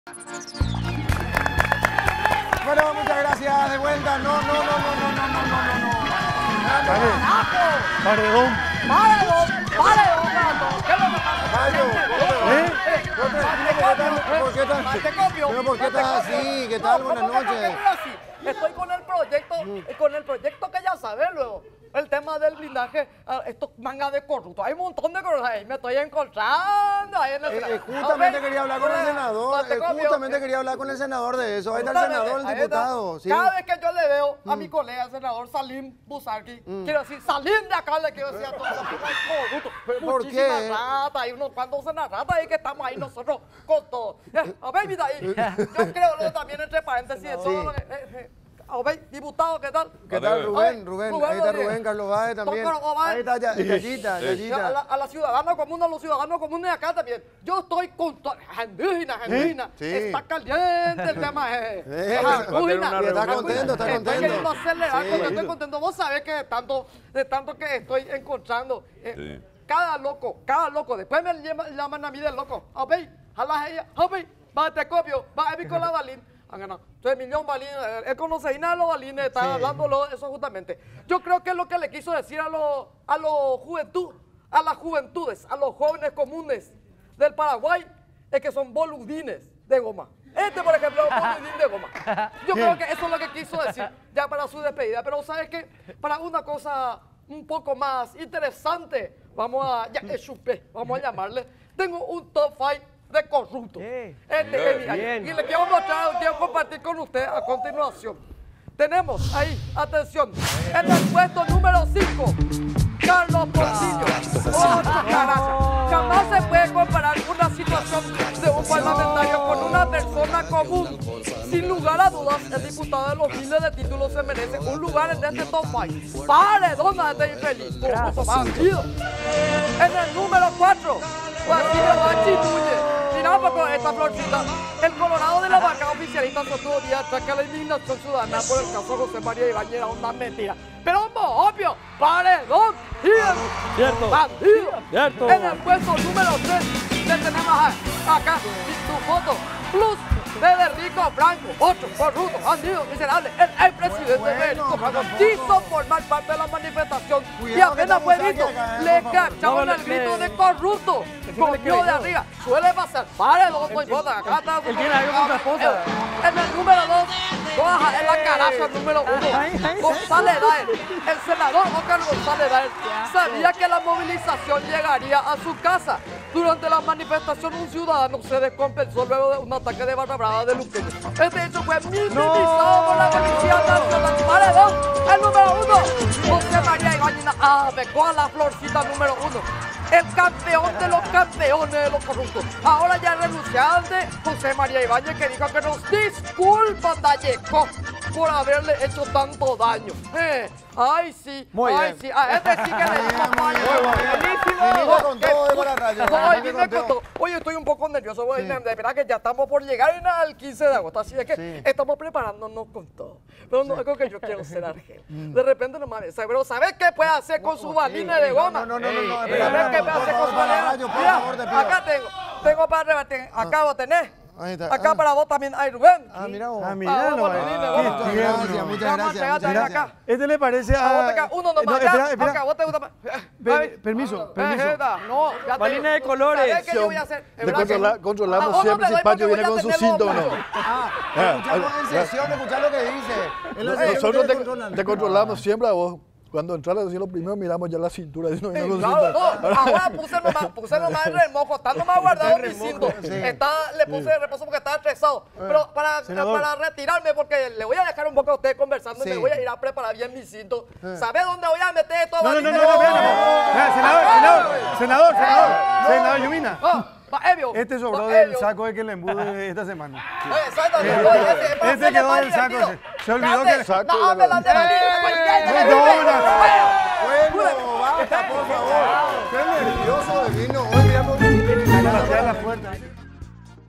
Top5 de políticos corruptos, presentado por Pararrayo Cubas
El polémico Pararrayo Cubas, irrumpió en el Bar, para presentarnos un exclusivo Top 5 de políticos corruptos ¡Es genial!